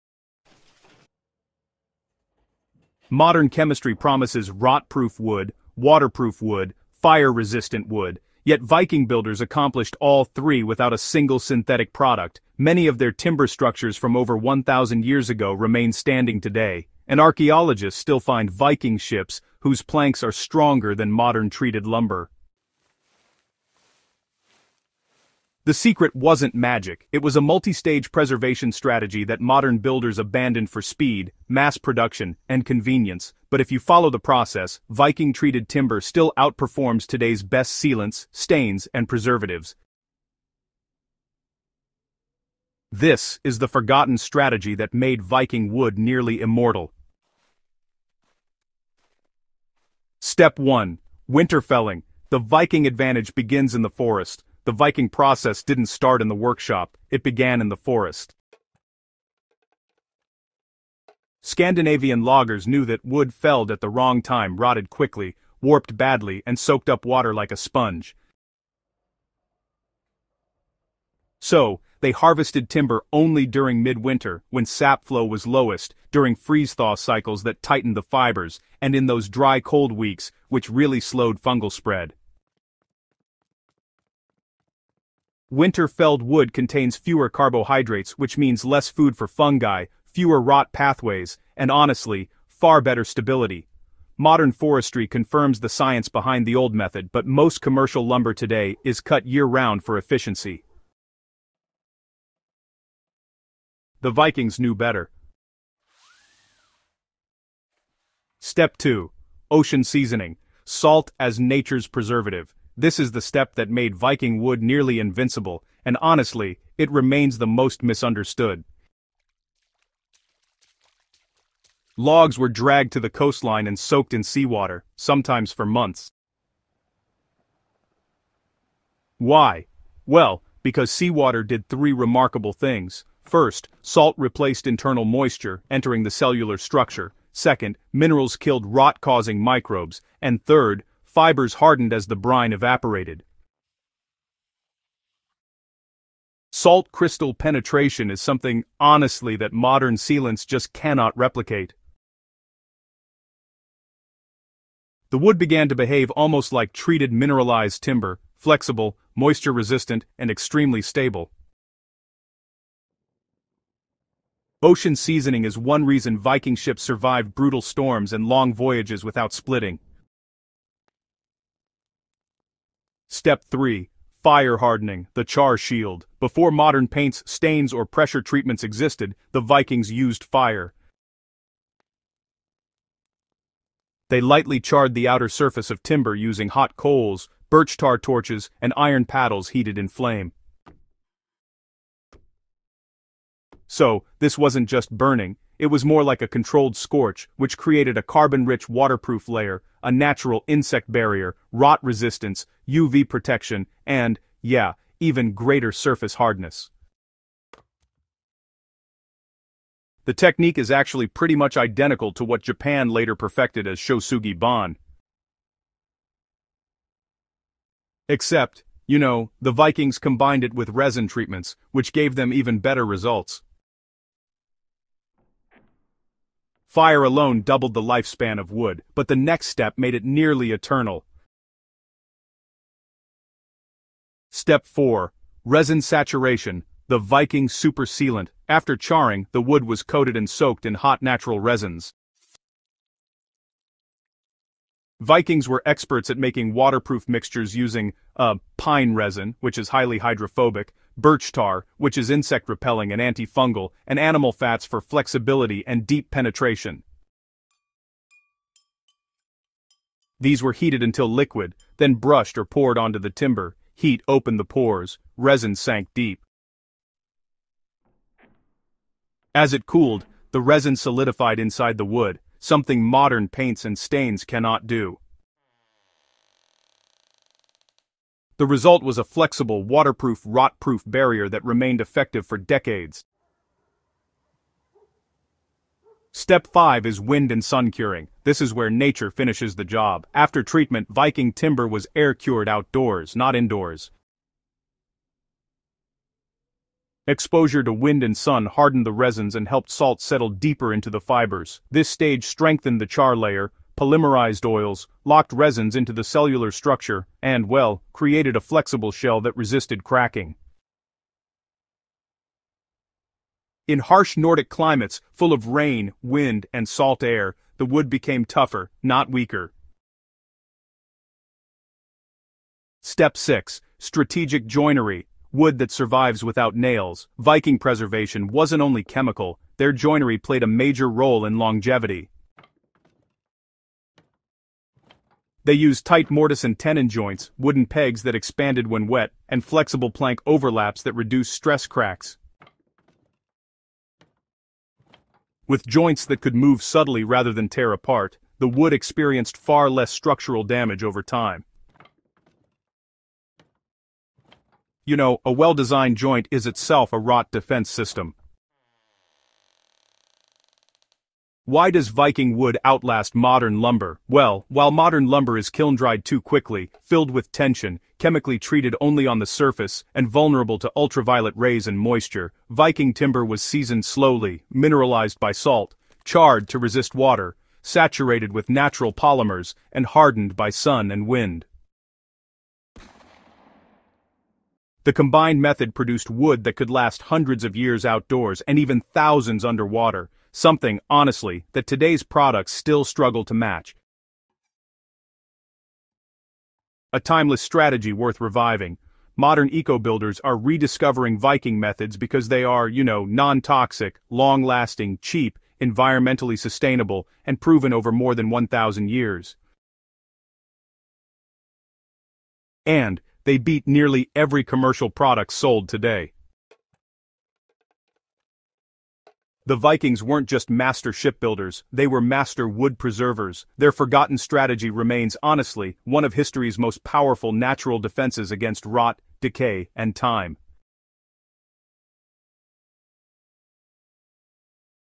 The Viking Preservation Strategy That Beats Today’s Best Products
Modern wood preservation relies heavily on harsh, chemical-laden products promising decades of defense. Yet, these often fall short when compared to the astonishing longevity of Viking-era timber—the longships and stave churches that have defied rot and decay for over a thousand years in one of the world's most unforgiving climates.

The secret wasn't a single ingredient, but a sophisticated, multi-stage Viking Preservation Strategy. It combined deep knowledge of wood science, specialized preparation, and a simple, powerful natural coating. This holistic approach created a resilience that actually beats today’s best commercial products in terms of sustained, non-toxic, centuries-long protection.

In this video, we dissect the complete Viking strategy for wood preservation:

🌳 The Material Maximization: The specific, systematic method of wood selection and drying that prepared the timber internally for maximum rot defense.

🛠️ The Structural Layer: The ingenious, non-standard joinery and construction techniques that prevented water ingress and pooling, which are primary causes of decay.

🛡️ The Triple-Defense Coating: The specific natural oil and tar-based compound (the "black shield") applied in multiple stages to create a deep, durable barrier.

🔬 The Modern Failure: Why modern chemical treatments often fail over time compared to the Vikings' integrated, natural approach.

🏆 The Verdict: Scientific evidence supporting the superior long-term performance of the Viking method compared to today's best products.

Discover the comprehensive, sustainable, and superior Viking Preservation Strategy—lost knowledge that proves the ancient masters held the key to truly eternal wood.